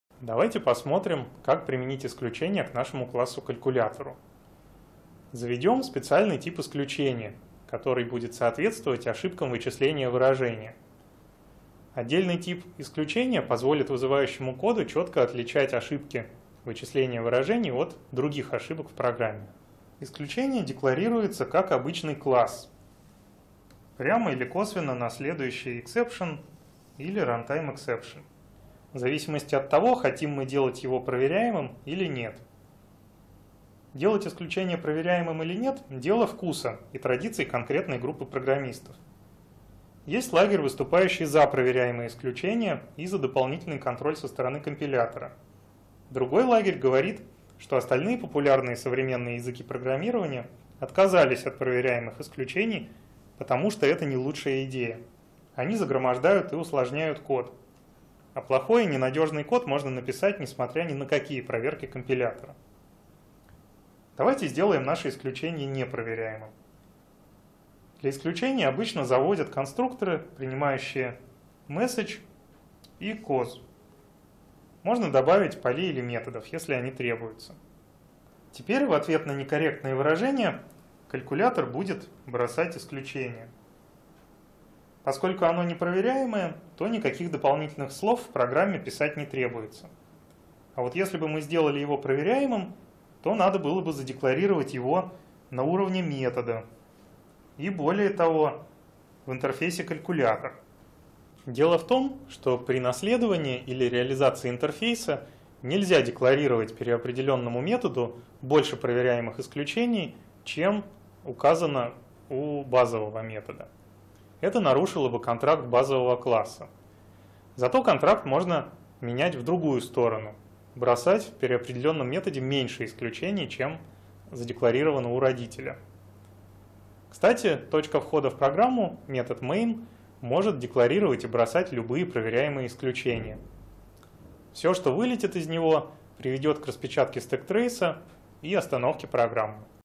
Базовый курс Java - #62 урок. Пример собственного типа исключения
Задача №1

Допустим, в программе используется следующее приведение типа:

A a = (A) b;
где A — имя какого-то класса, a и b — переменные.
Если компилятор сможет сразу определить, что такое приведение невозможно, то компиляция завершится ошибкой. Однако не всегда компилятор может это сделать, т.к. фактический объект, находящийся по ссылке b, на момент компиляции не известен. Поэтому иногда такой код может успешно скомпилироваться, но упасть с ошибкой во время исполнения.

Какое исключение выбросит JVM, если во время выполнения программы окажется, что ссылка b не может быть приведена к типу A?

Введите полное имя класса исключения, включая пакет, например, com.example.FooBarException

P.S. Эта задача предполагает проведение небольшого эксперимента. В лекциях нет ответа на этот вопрос.